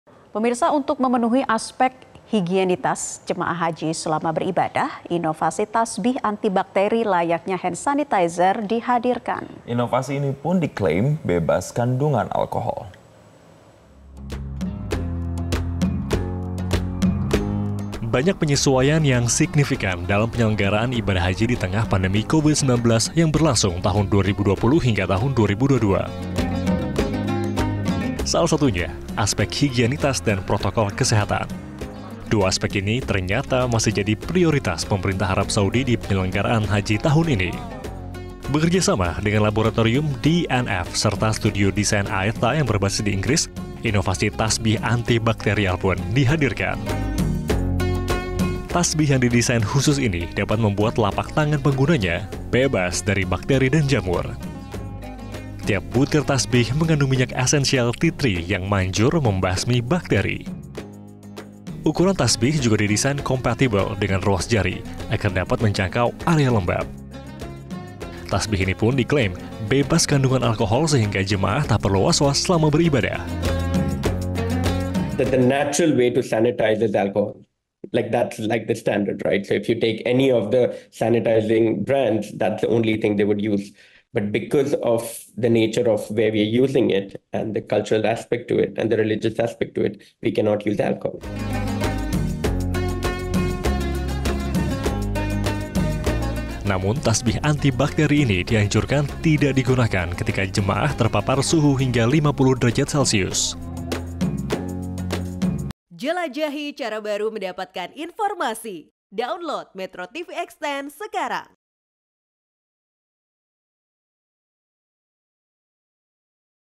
Inovasi Tasbih Anti Bakteri
MetroTV, Untuk memenuhi aspek higienitas jemaah haji selama beribadah, inovasi tasbih anti bakteri layaknya Hand Santizier dihadirkan. Inovasi ini pun diklaim bebas kandungan Alkohol.